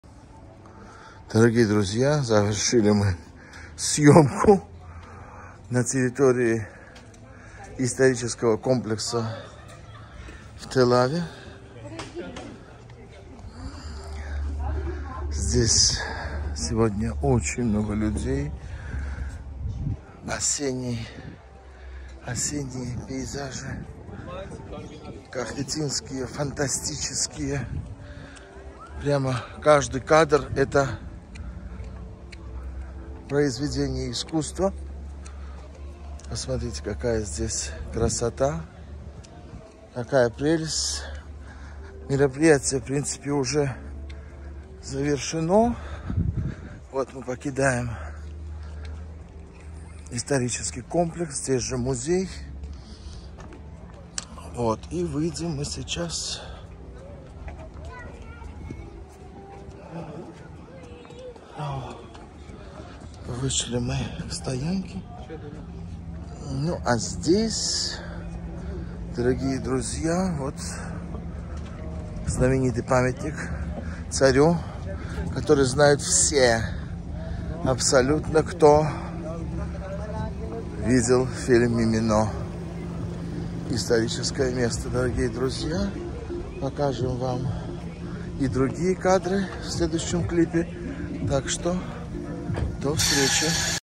Грузия. Кахетия. Телави. Исторический центр города.
В крепости «Батонис цихе» проходили праздничные мероприятия. Телави 7 ноября отмечал именины и день рождения Царя Ираклия Второго (Эрекле меоре). Вокруг осенние пейзажи. Красота. После съёмок репортажа отправляемся на небольшую прогулку по городу!

Наших зрителей ждет несколько материалов из Кахетии. Так что 🍇❤️😍 приятного путешествия в мир вина, виноделия и гостеприимства!